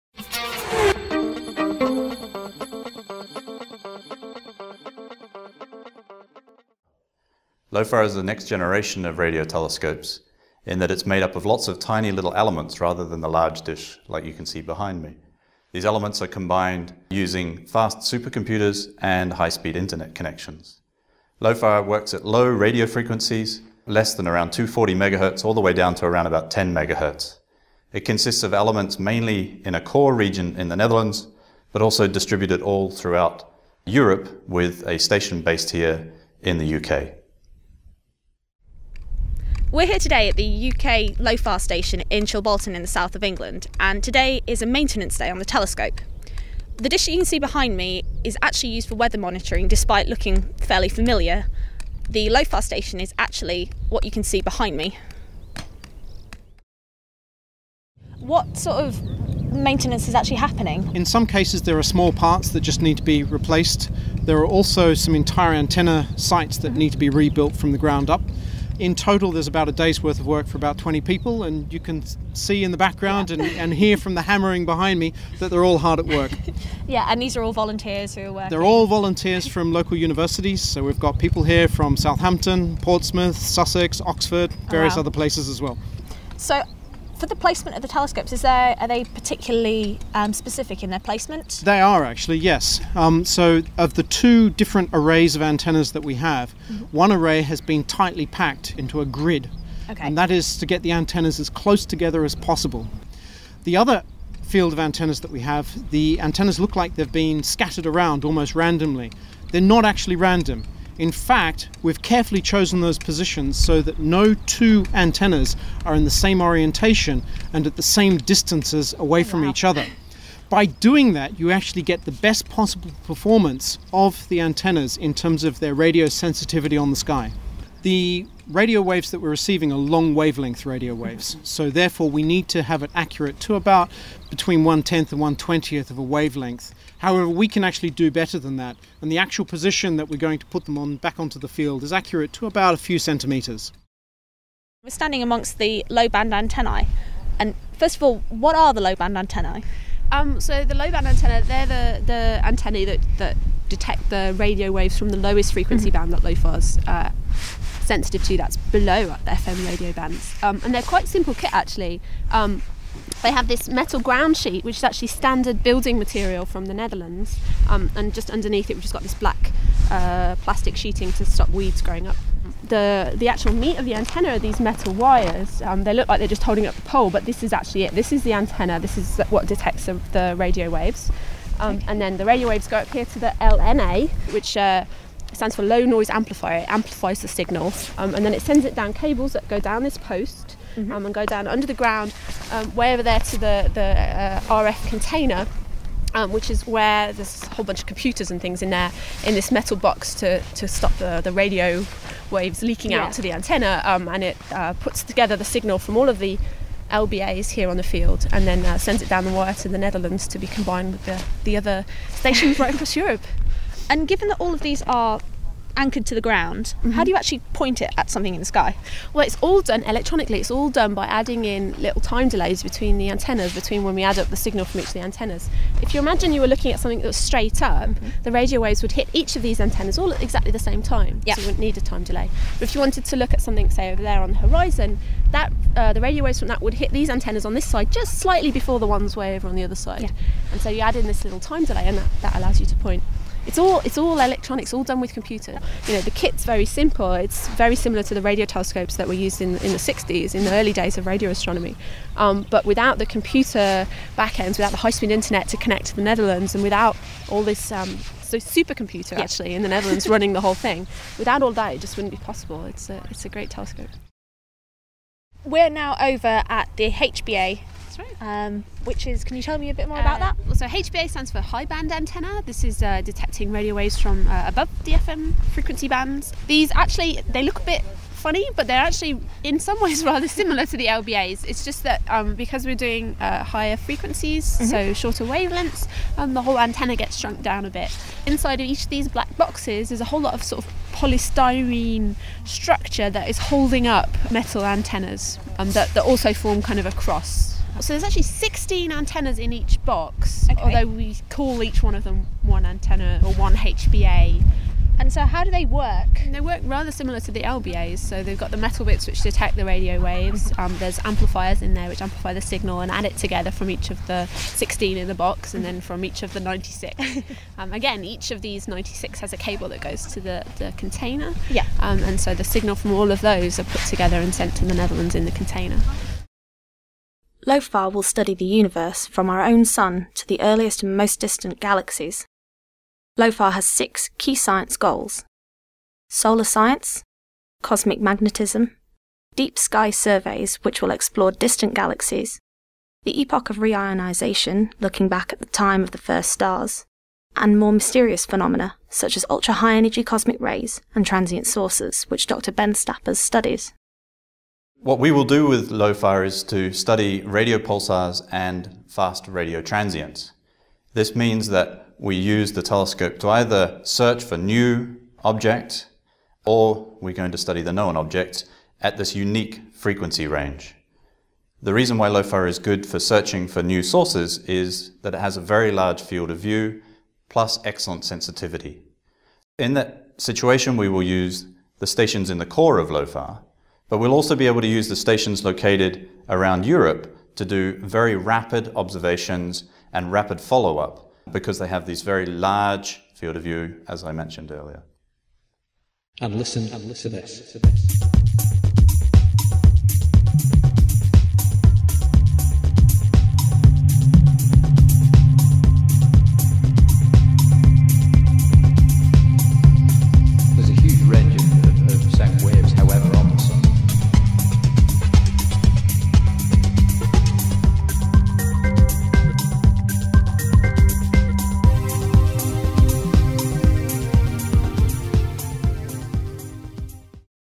jodcast lofar
In this video, three Jodcasters go to watch the UK LOFAR station being repaired. Christina finds out about this unusual-looking radio telescope, which is observing the low frequencies of the Universe better than ever before.